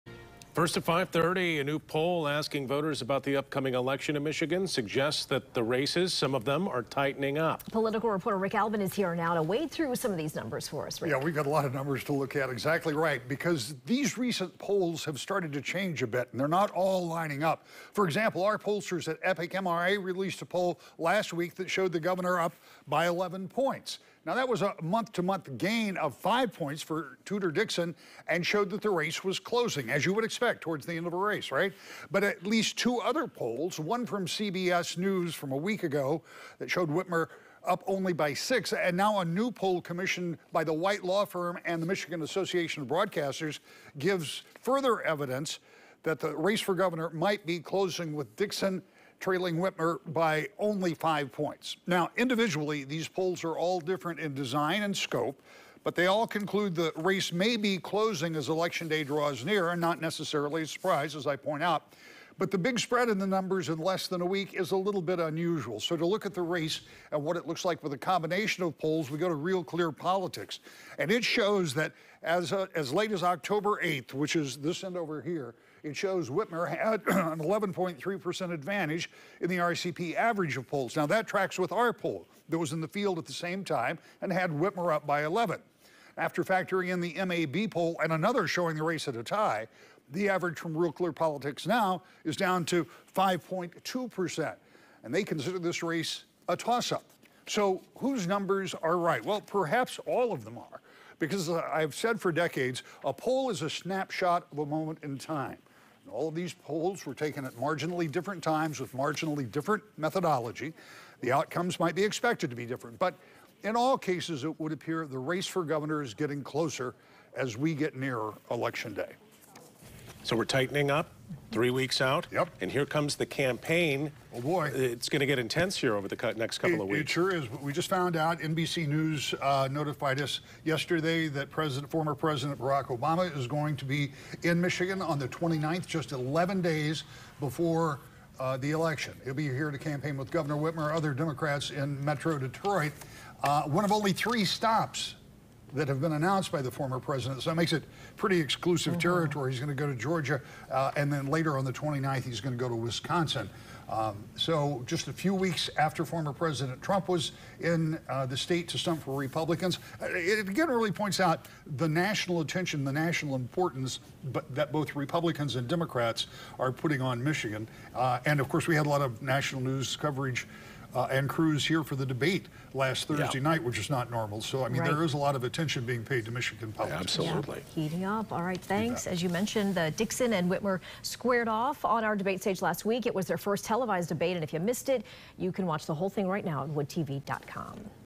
New polls show governor's race tightening up
New poll results suggest that the race for governor between Gretchen Whitmer and Tudor Dixon may be closer than previously expected.